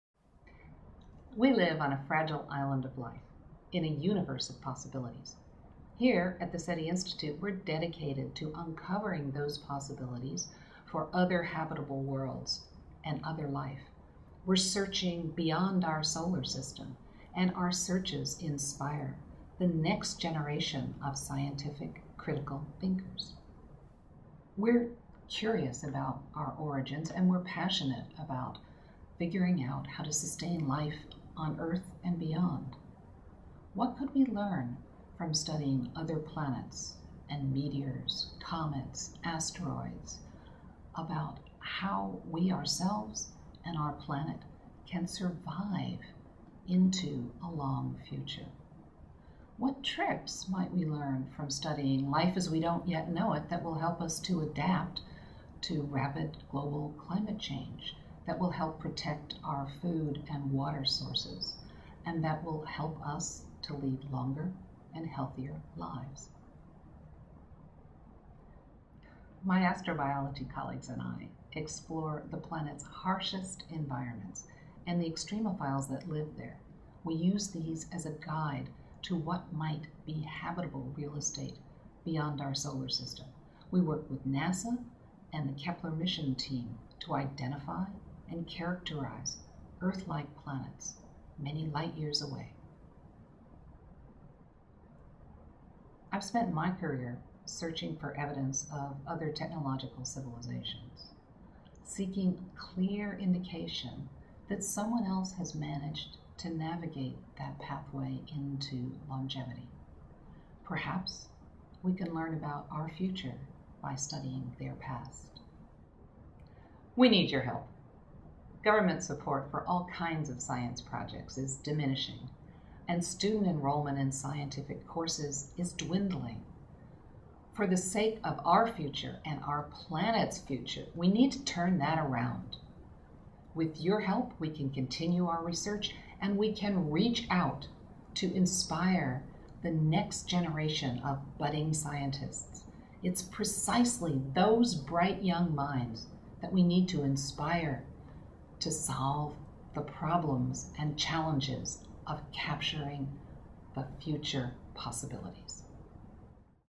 Jill Tarter, Bernard Oliver Chair for SETI, SETI Institute
Jill Tarter talks about her experiences, and the need to support the future of scientific research.

With government funding of science programs at an all-time low, the future of our research and mankind's continued search for cosmic knowledge is under threat.

and unite with curious thinkers across the globe in helping to expand our research and continue the search for answers on Earth and beyond. You can be a part of pushing the boundaries of scientific knowledge with us by donating today.